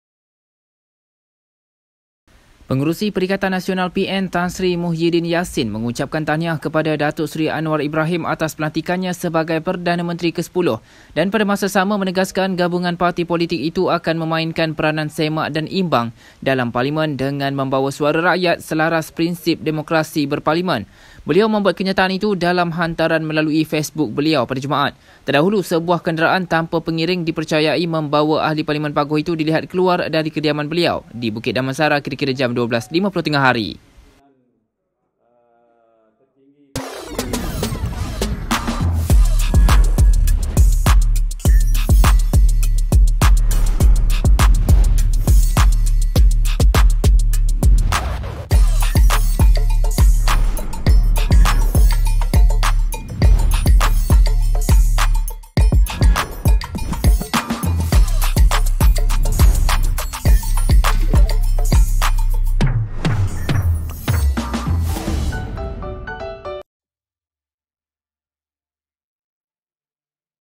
Tahniah Anwar, PN akan mainkan peranan semak dan imbang dalam Parlimen - Muhyiddin
KUALA LUMPUR: Pengerusi Perikatan Nasional (PN), Tan Sri Muhyiddin Yassin, mengucapkan tahniah kepada Datuk Seri Anwar Ibrahim atas pelantikannya sebagai Perdana Menteri ke-10, dan pada masa sama menegaskan gabungan parti politik itu akan memainkan peranan semak dan imbang dalam Parlimen dengan membawa suara rakyat selaras prinsip demokrasi berparlimen.